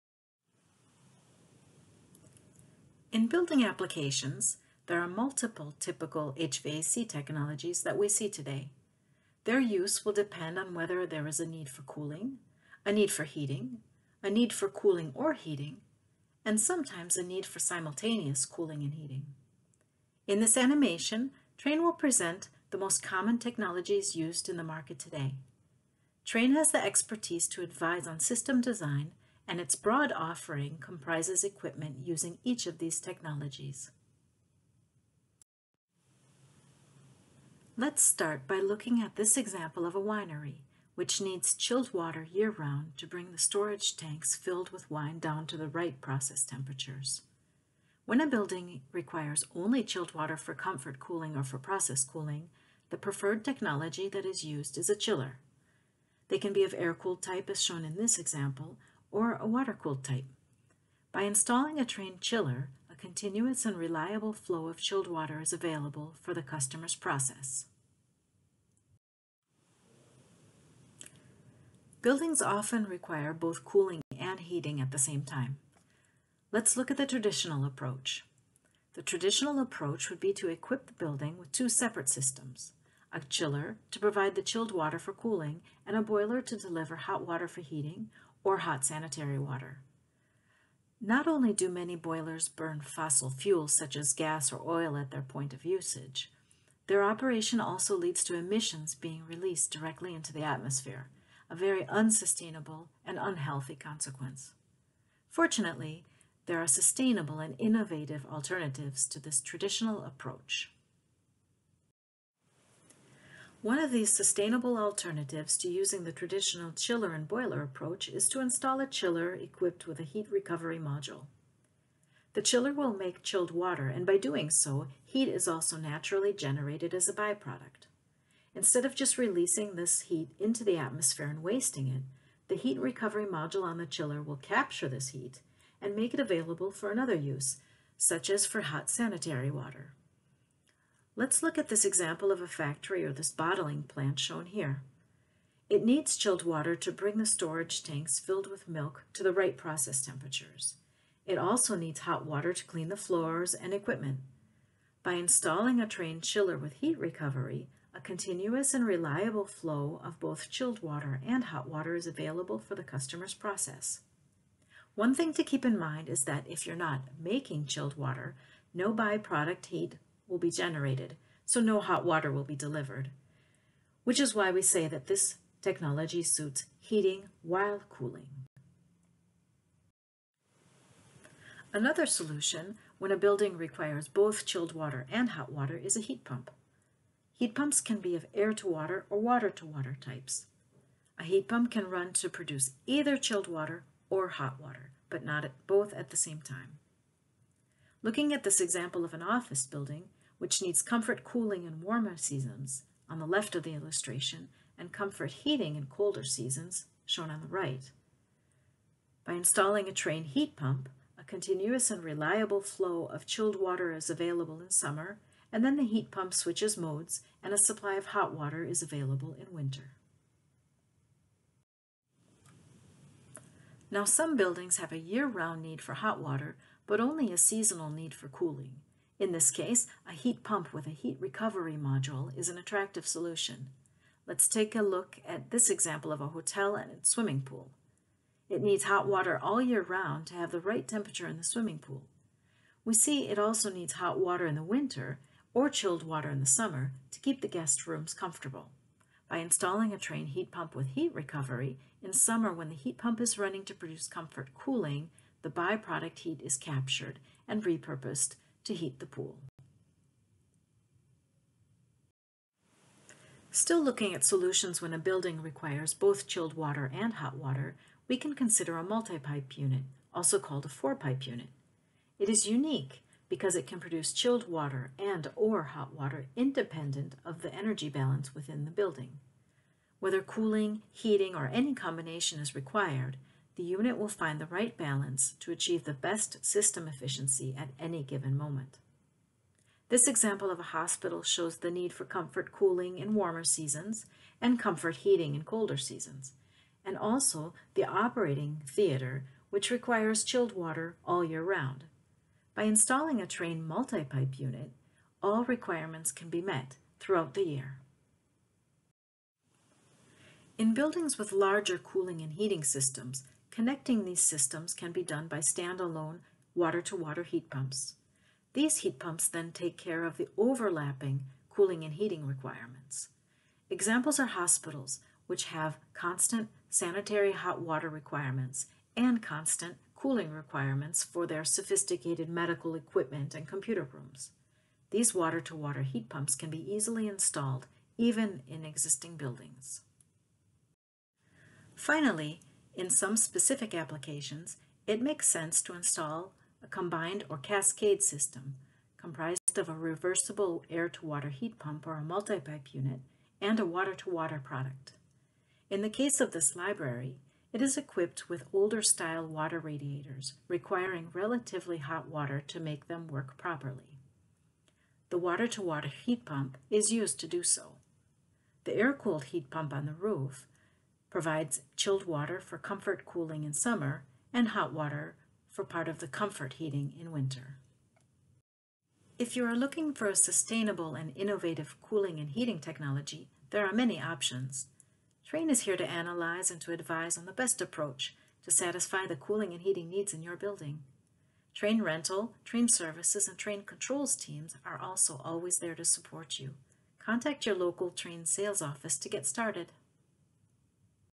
HVAC Technologies in Applications
Discover the different HVAC technologies that can be applied, depending on the cooling and heating needs of buildings.
A short, informative video sharing descriptions of each of the following sustainable solutions:
Chiller, Chiller with Heat Recovery, Heat Pump, Heat pump with Heat recovery, Multi-pipe Unit, Water-to-Water Heat Pump, or a Cascade system.